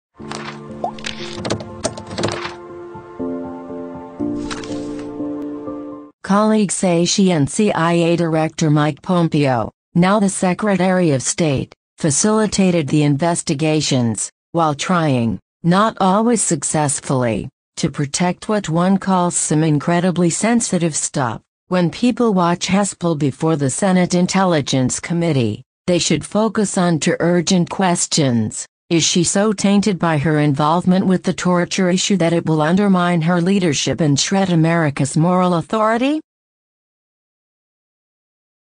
Gina Haspel is tainted by her torture involvement. But she understands Russia.
Colleagues say she and CIA Director Mike Pompeo (now the secretary of state) facilitated the investigations, while trying, not always successfully, to protect what one calls “some incredibly sensitive stuff.”
When people watch Haspel before the Senate Intelligence Committee, they should focus on two urgent questions: Is she so tainted by her involvement with the torture issue that it will undermine her leadership and shred America’s moral authority?